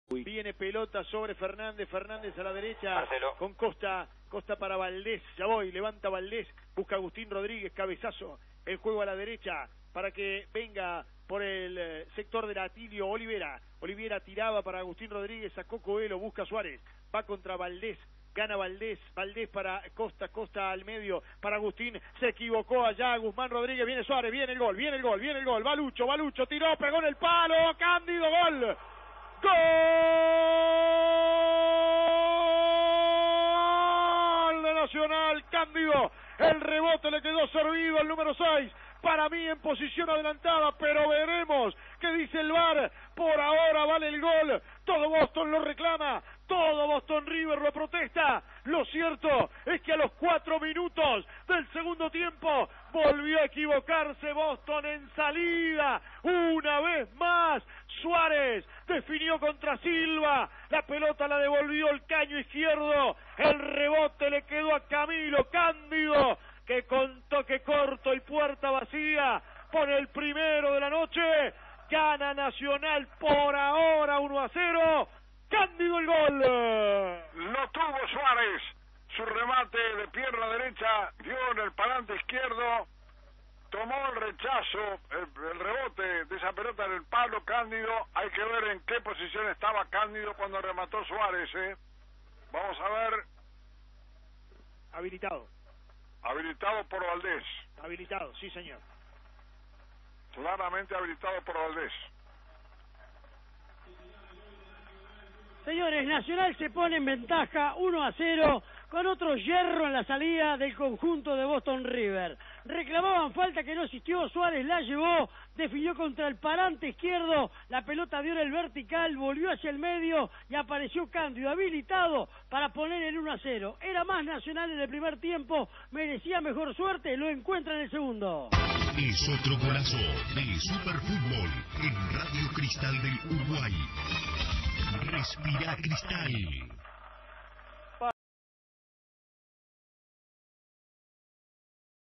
Nacional 1 Boston River 0 - Clausura 2022 (por Marcelo Sanzó)
El gol del partido, por Marcelo Sanzó junto a Jorge da Silveira y Claudio Veiga para CX 147 Radio Cristal del Uruguay 1470 AM.